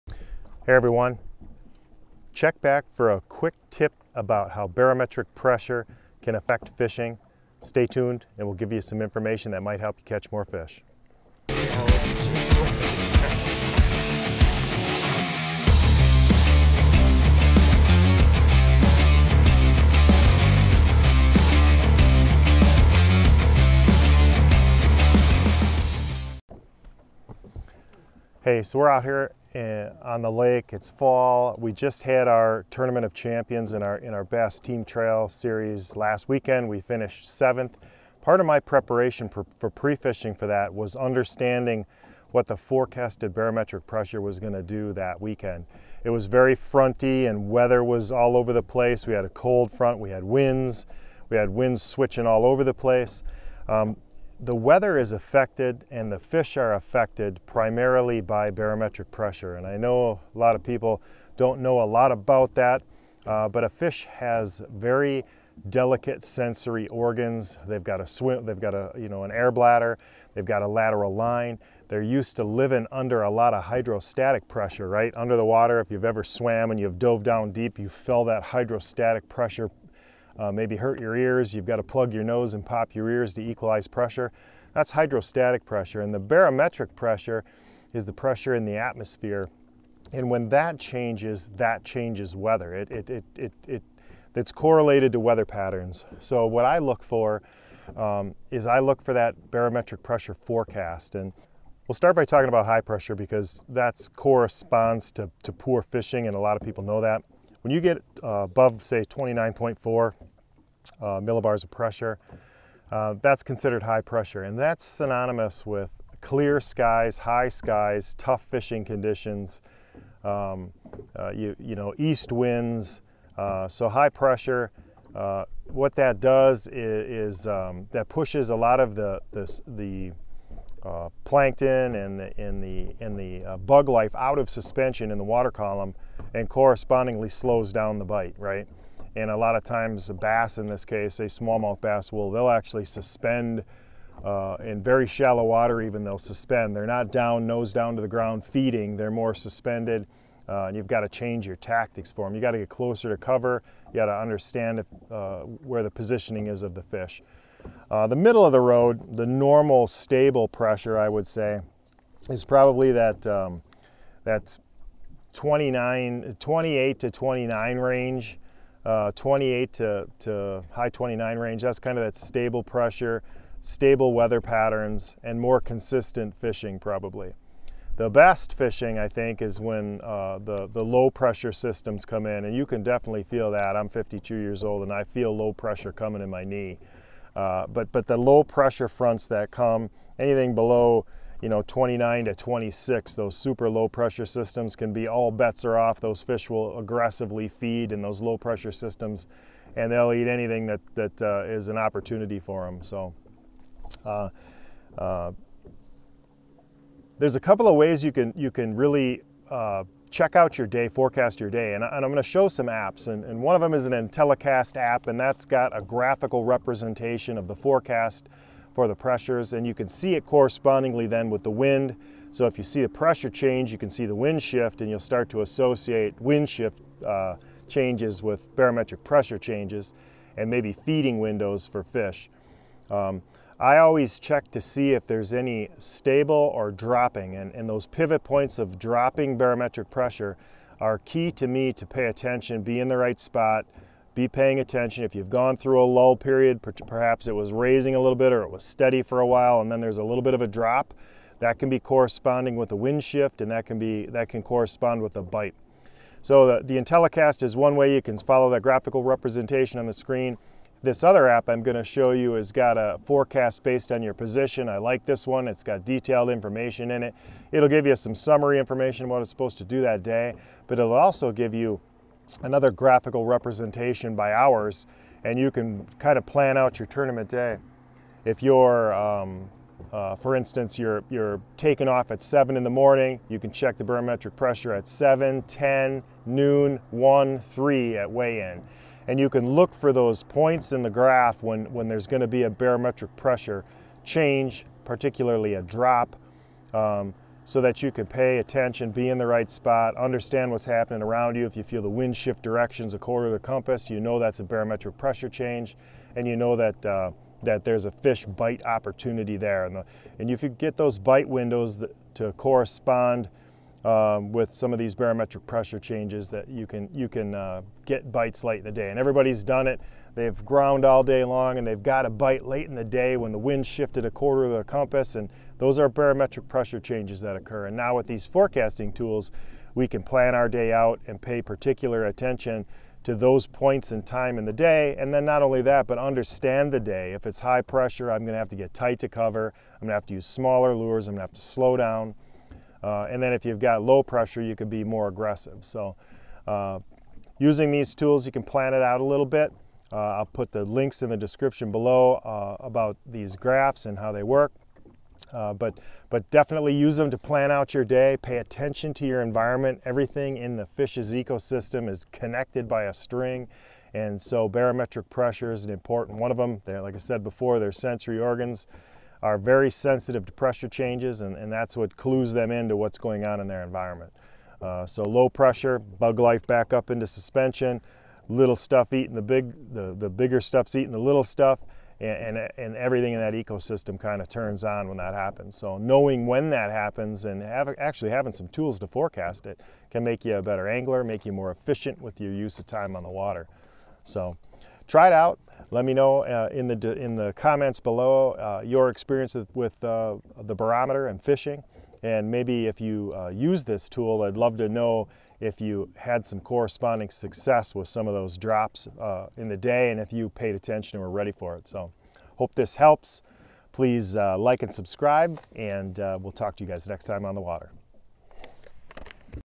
Barometric Pressure Change Alert
In this video I go over some barometric pressure change alert ideas that can help you plan for your next tournament of fishing outing.  Understanding how barometric pressure affects fish and when those barometric pressure changes will change, can really give you an advantage when conditions change. You will get some good information and tools to better plan your next fishing outing.

Barometric Pressure Location Forecast Application.  Run from the browser of your phone, or create a shortcut on your phone's homepage with this link:

If you enjoy the content, please consider helping me out.  Visit my membership page!